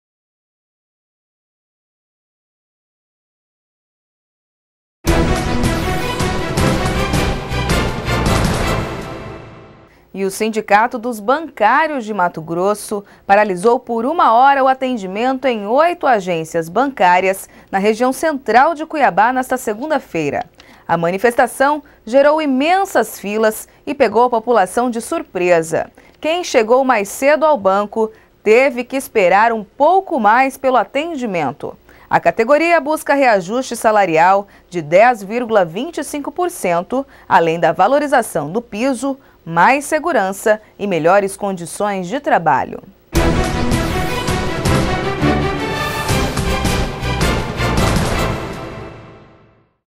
Paralisação de bancários pega população de surpresa
O Sindicato dos Bancários de Mato Grosso (SEEB-MT) paralisou por uma hora o atendimento em oito agências bancárias da região central de Cuiabá. A manifestação gerou imensas filas e pegou a população de surpresa. Quem chegou mais cedo ao banco, teve que esperar um pouco mais pelo atendimento. A categoria busca reajuste salarial de 10,25%, além da valorização do piso, mais segurança e melhores condições de trabalho..